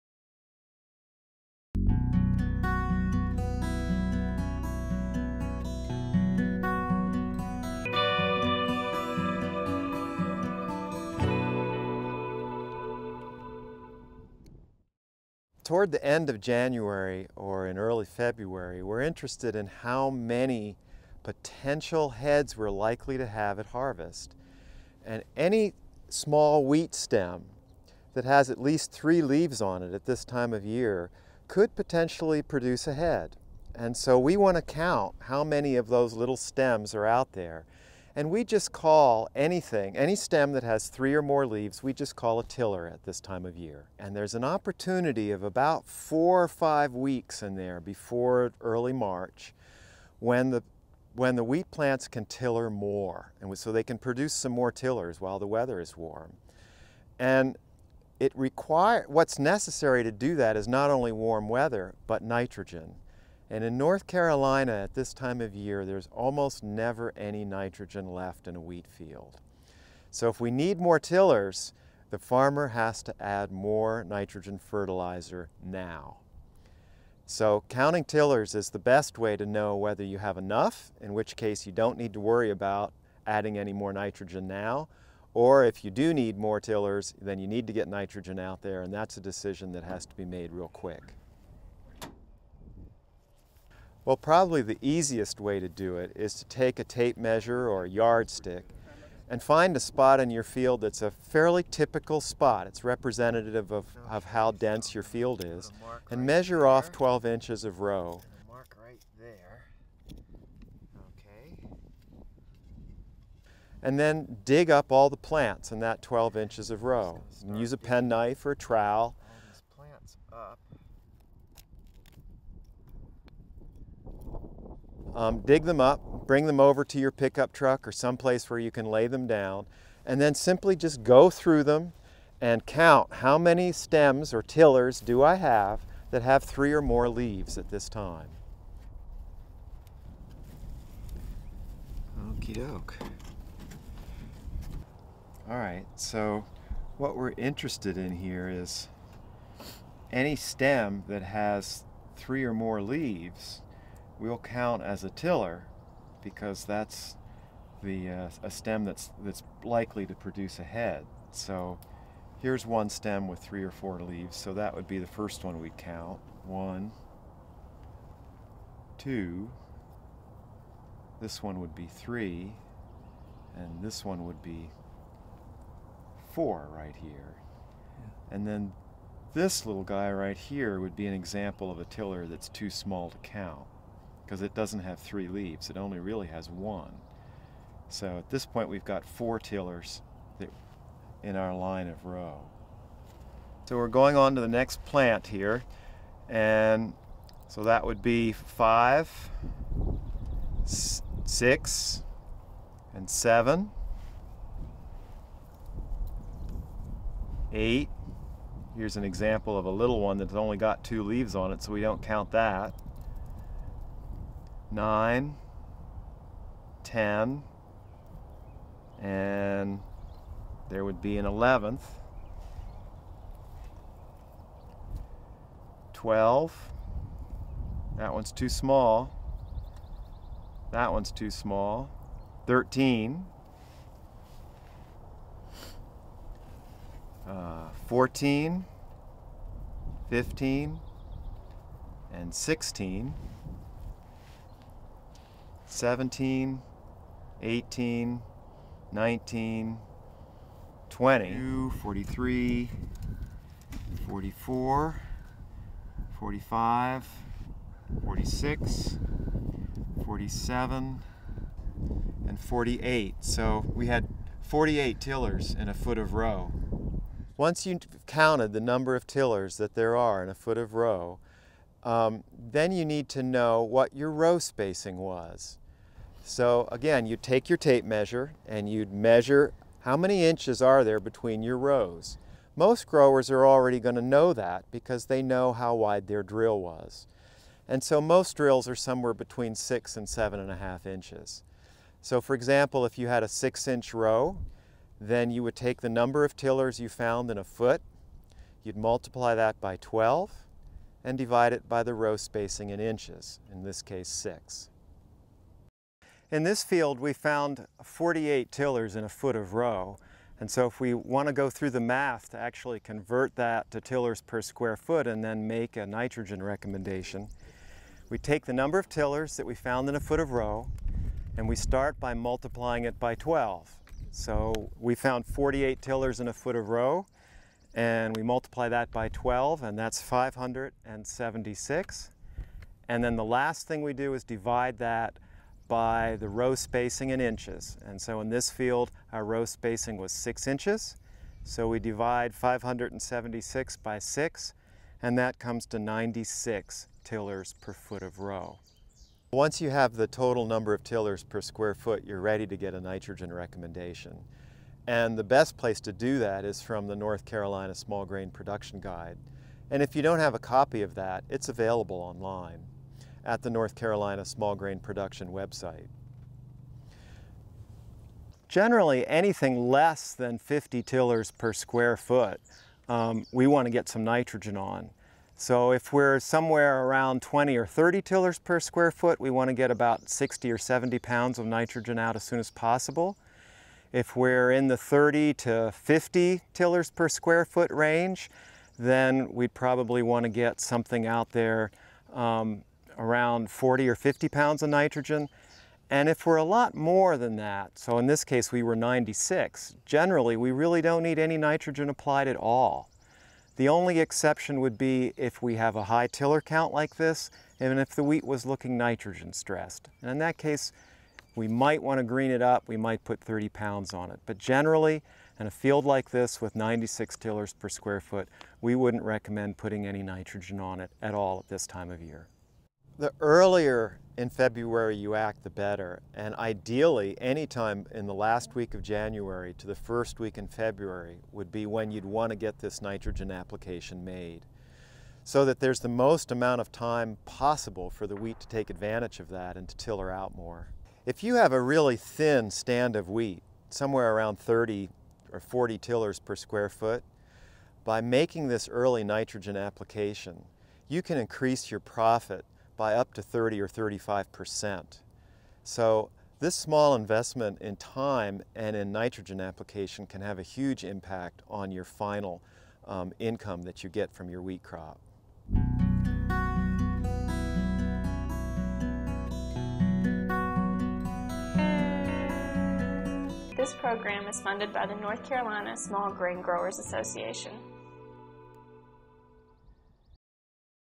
Counting Tillers To Optimize Spring Nitrogen Rates & Timing
A step by step video on how to determine tiller density at growth stage 25 (late January to early February), to optimize Nitrogen rates and timing in soft red winter wheat.